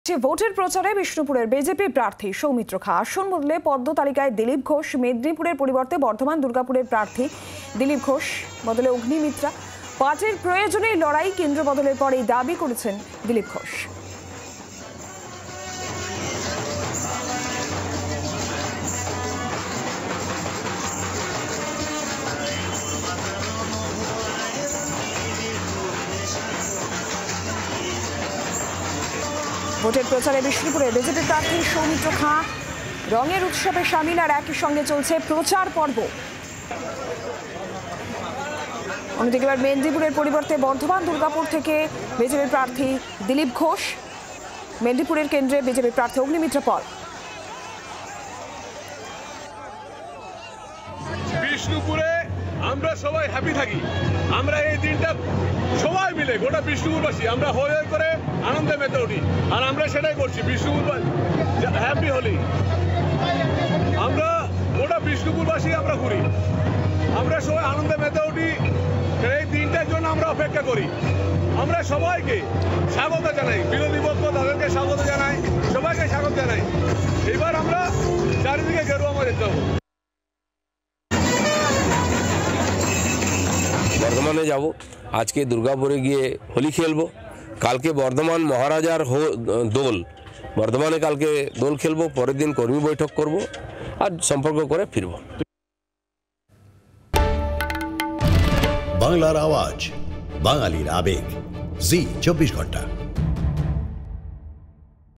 Lok Sabha Election: রঙের উত্সবের মধ্যেই জনসংযোগ সৌমিত্র-দিলীপের! | Zee 24 Ghanta
Lok Sabha Election: রঙের উত্সবের মধ্যেই জনসংযোগ সৌমিত্র-দিলীপের! দেখুন কী বললেন বিষ্ণুপুরের বিজেপি প্রার্থী সৌমিত্র খাঁ | Zee 24 Ghanta